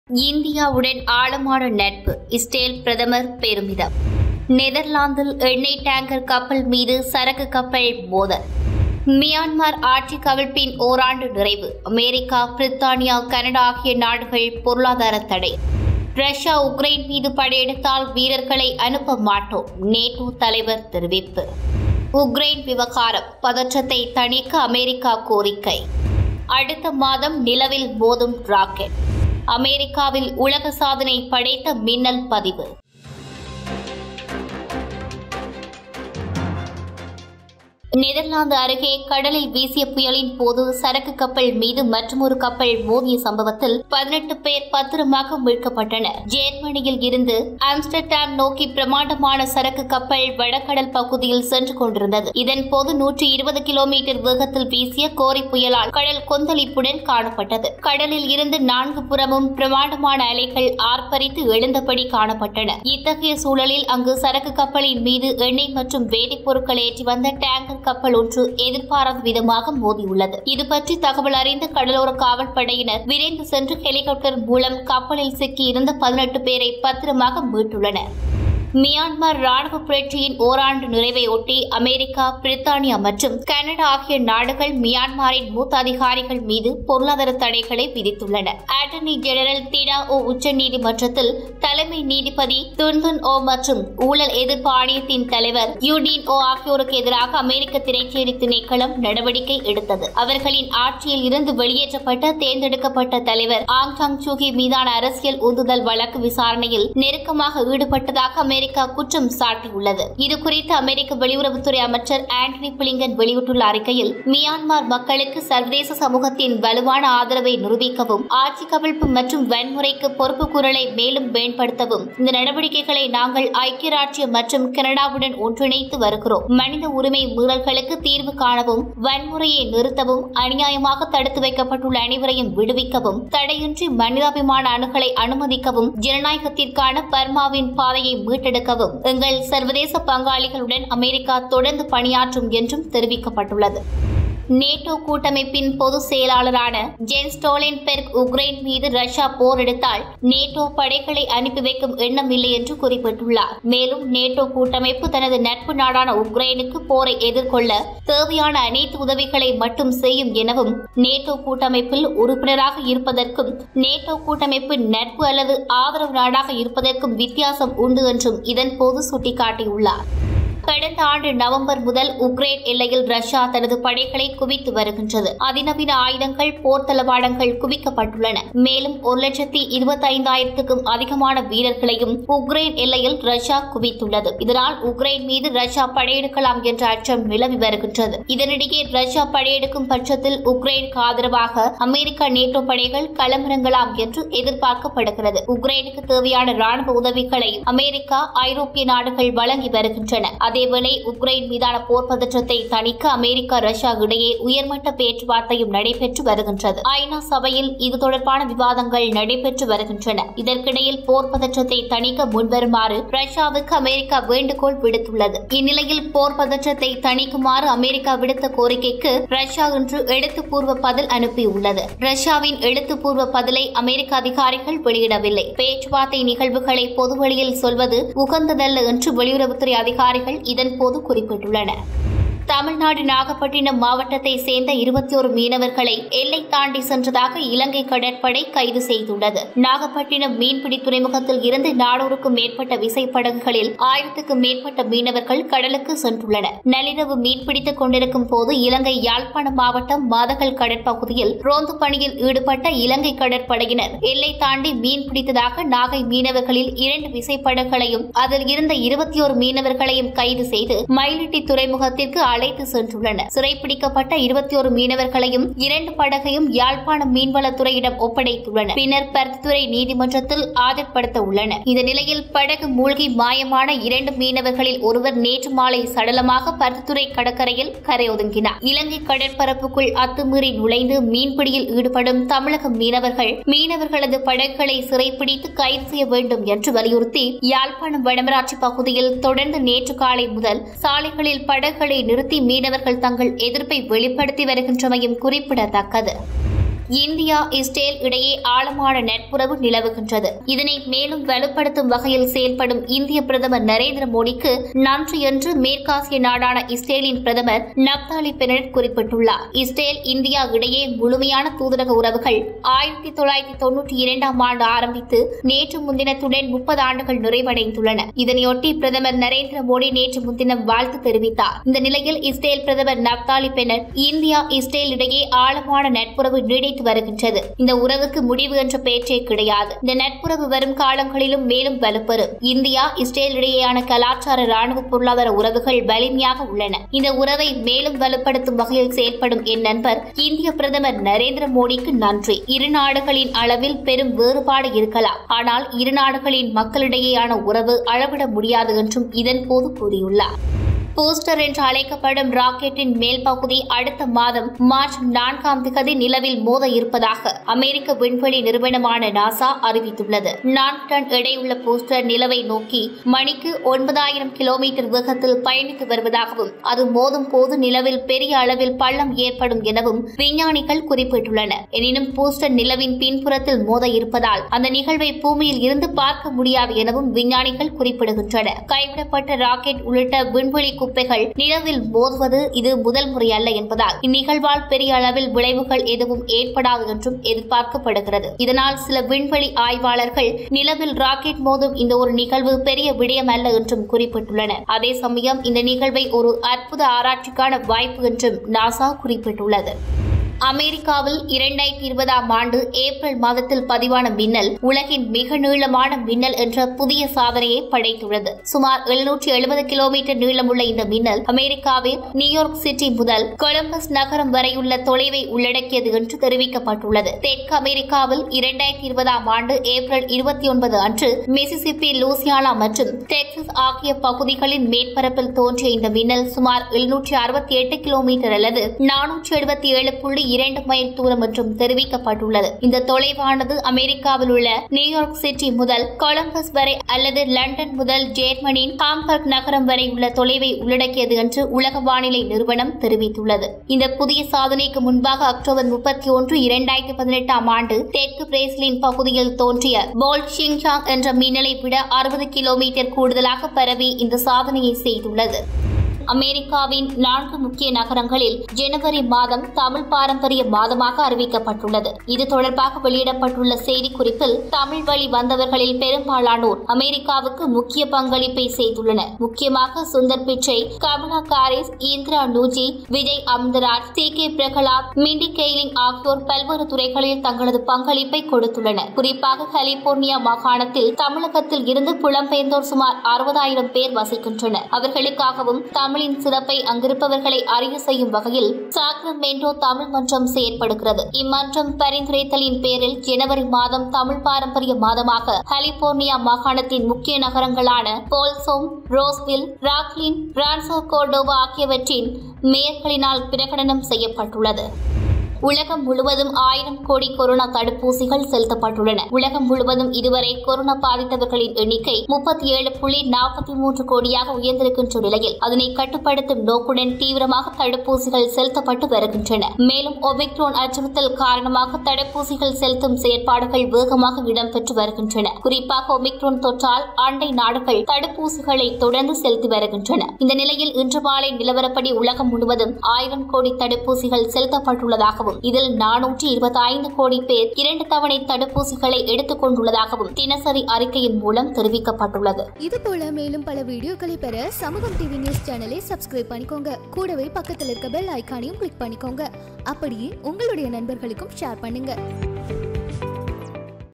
உலக செய்திகள் - 1.2.2022 || World News Tamil || Cargo ship collides with an oil tanker
-Deep friendship with India: Israeli PM proud
-Netherlands: Cargo ship collides with an oil tanker
One year after the coup in Myanmar: US, Britain and Canada impose sanctions!
-NATO leader will not send troops if Russia invades Ukraine !!
-Ukraine affair: US demand to ease tensions
-Rocket to hit the moon next month
-World record-breaking lightning record in the United States!